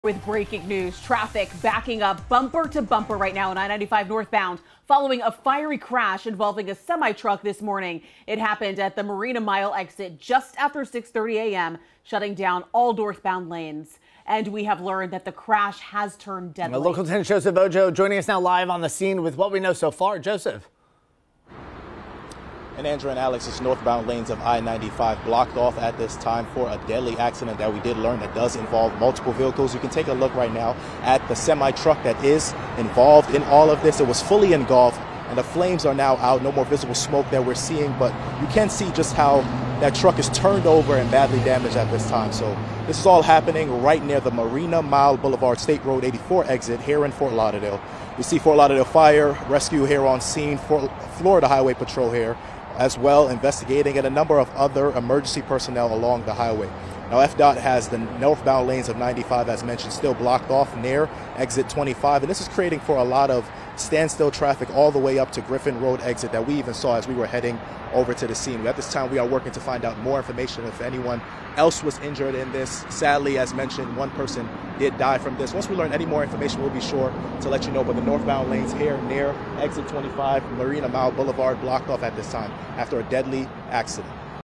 Fatal crash blocks I-95 NB in Fort Lauderdale
A fatal multiple-vehicle crash was blocking Interstate 95 northbound lanes in Fort Lauderdale.